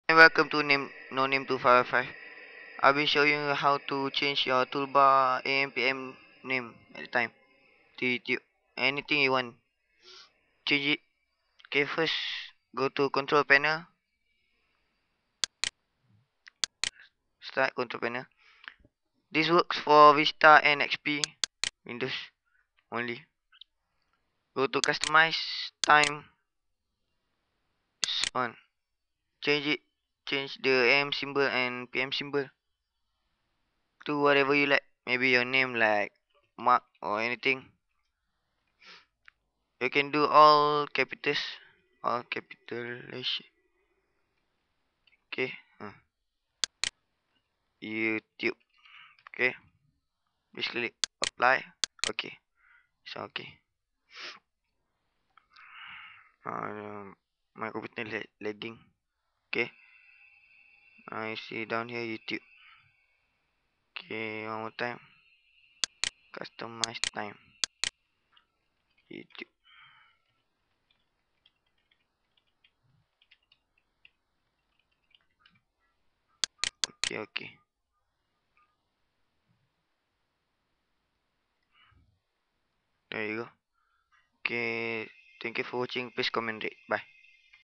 How to change the toolbar to your name for xp & vista
PLEASE READ!!

I'll be showing you how to change AM & PM next to your time in the toolbar to anything you want.

Please comment and rate. if you have any question just hit me.